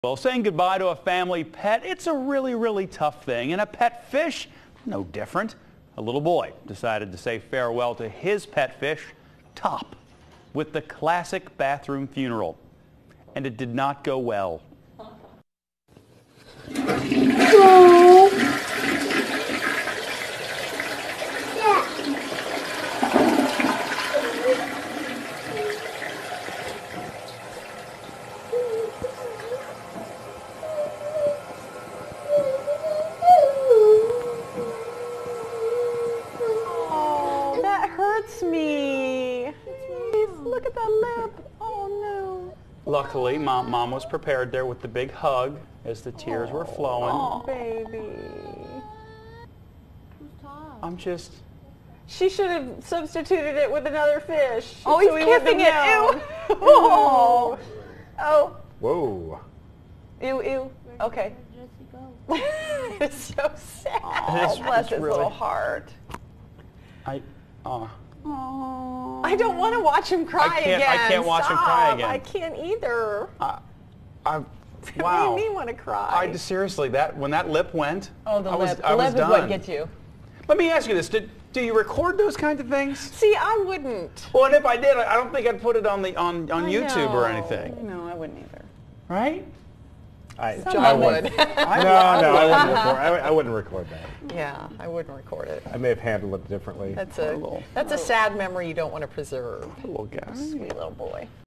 Young boy has sad farewell to pet goldfish
(YouTube) -- Saying goodbye to a family pet is one of the hardest things in life, and pet fish are no different.    A little boy decided to say farewell to his pet fish "Top" with the classic bathroom funeral.    It didn't go so well.